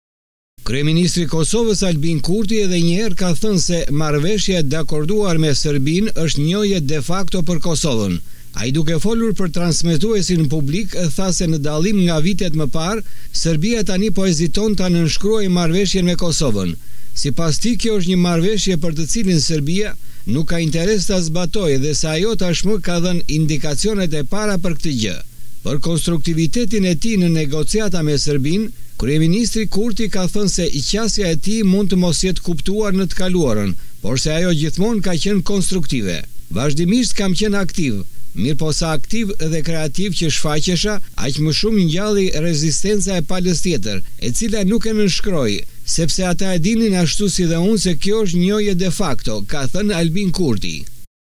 Kurti ‘këndon’ refrenin dhe mburret/ Marrëveshja, njohje de-facto për Kosovën
Kryeministri i Kosovës, Albin Kurti edhe një herë ka thënë se marrëveshja e dakorduar me Serbinë është njohje de-facto për Kosovën.
Ai, duke folur për transmetuesin publik, ka thënë se në dallim nga vitet më parë, Serbia tani po heziton të nënshkruajë marrëveshje me Kosovën.
Sipas tij, kjo është një marrëveshje të cilën Serbia nuk ka interes ta zbatojë dhe se ajo tashmë ka dhënë indikacionet e para për këtë gjë.
Për konstruktivitetin e tij në negociata me Serbinë, Kurti ka thënë se qasja e tij mund të mos jetë kuptuar në të kaluarën por se ajo gjithmonë ka qenë konstruktive.
“Vazhdimisht kam qenë aktiv, mirëpo sa më aktiv dhe kreativ që shfaqesha, aq më shumë ngjallej rezistenca e palës tjetër, e cila nuk e nënshkroi, sepse edhe ata e dinin sikur unë se kjo është njohje de-facto”, ka thënë Kurti.

𝐀𝐮𝐭𝐨𝐫 : NTV